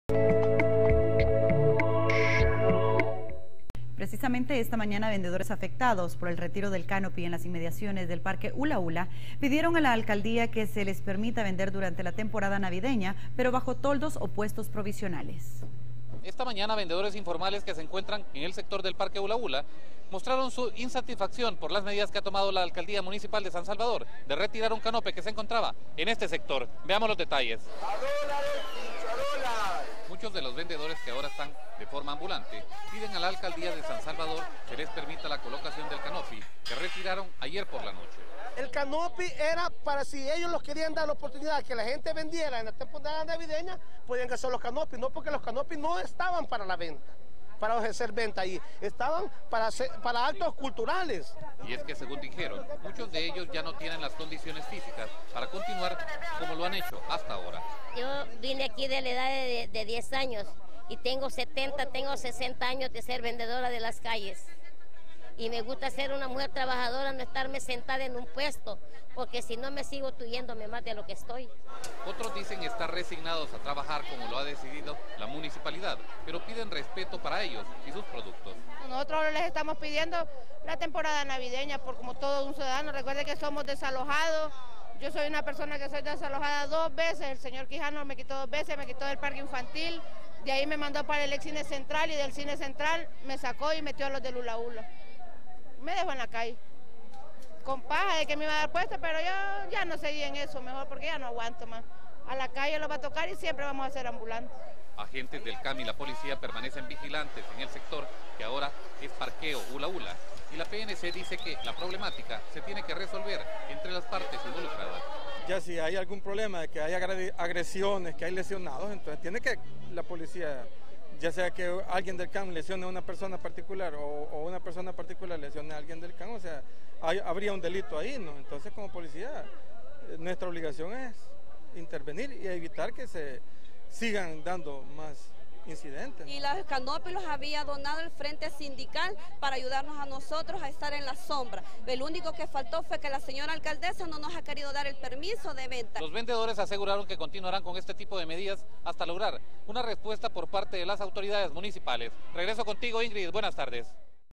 Vendedores exigen vender bajo toldos o puestos provisionales
Esta mañana vendedores afectados por el retiro del canopies en las inmediaciones del parque Hula - Hula, pidieron a la Alcaldía que se les permita vender durante la temporada navideña, pero bajo toldos o puestos provisionales.